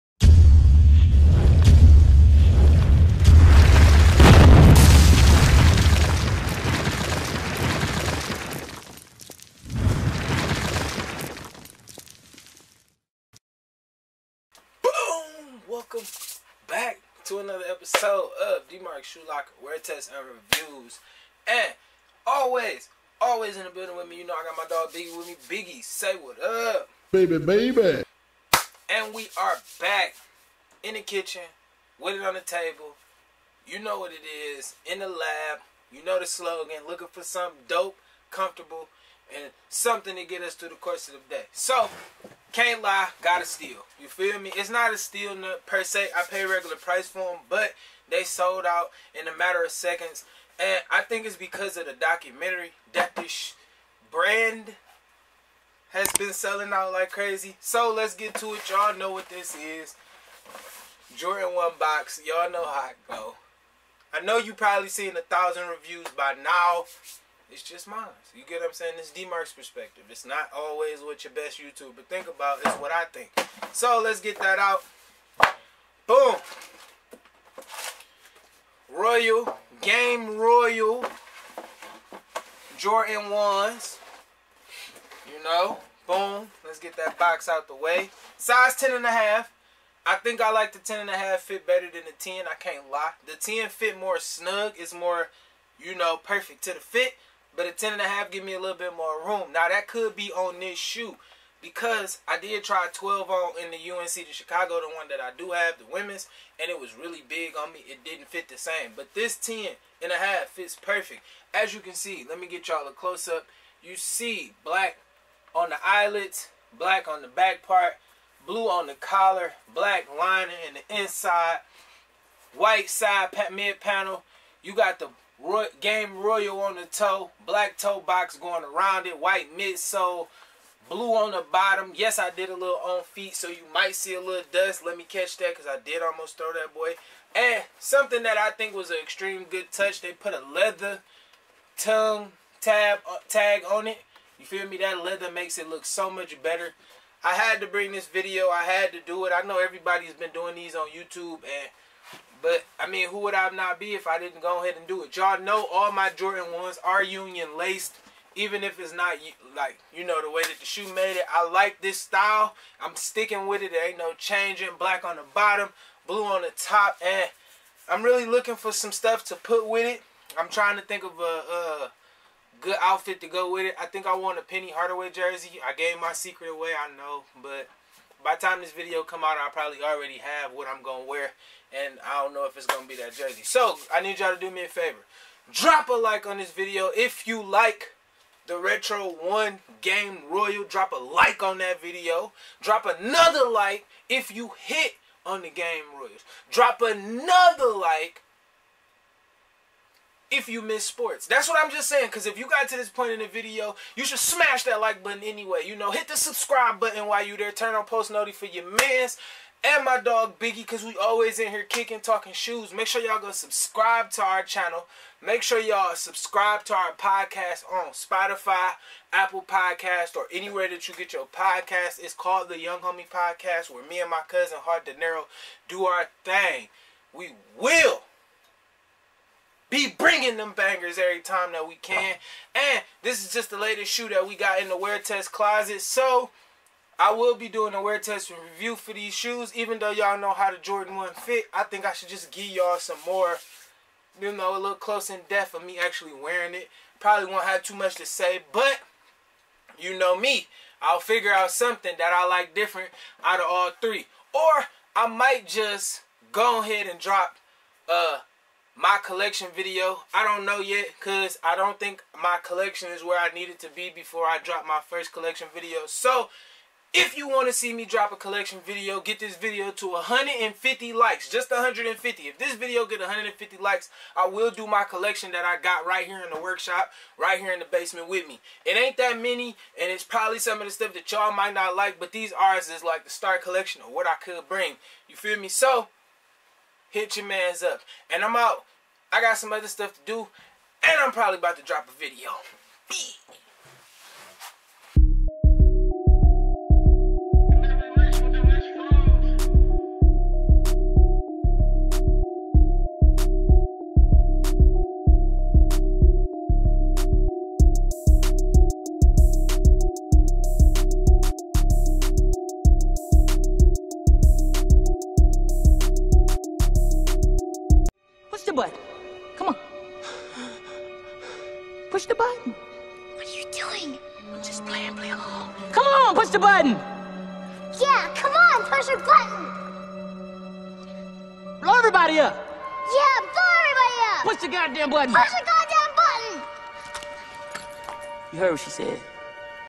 Do Not Buy The Jordan 1 (Game Royal) Into After You Watch This
On Today Episode We Go Over The Air Jordan 1 (Game Royal)
Let Us Know How U Feel Down In The Comments About The Air Jordan 1 (Game Royal) Thanks For Rocking With Ya Mans.